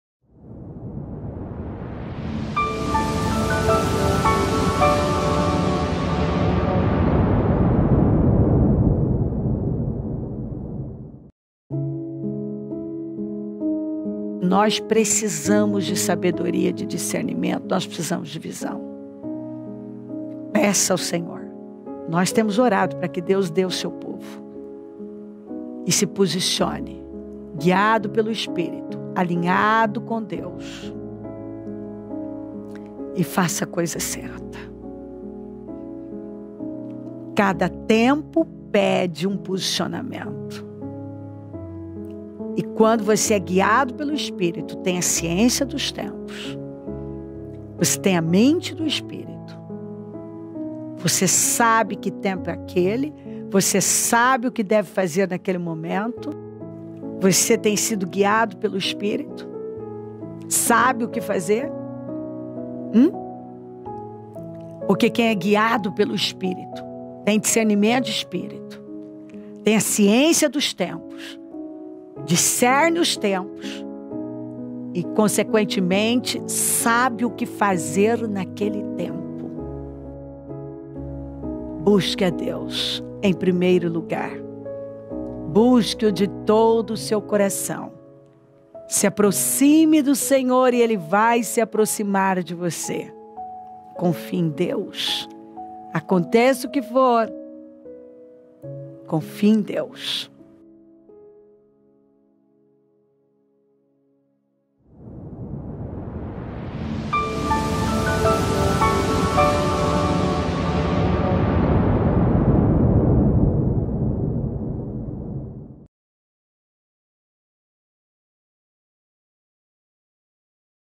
Série Famintos por Deus EP 212| ALINHADOS COM DEUS| Bispa Cléo
Trecho extraído de: Alinhe-se com os planos de Deus! __ Mudança de Vida Hoje

✝️  Seja um semeador na obra de Deus!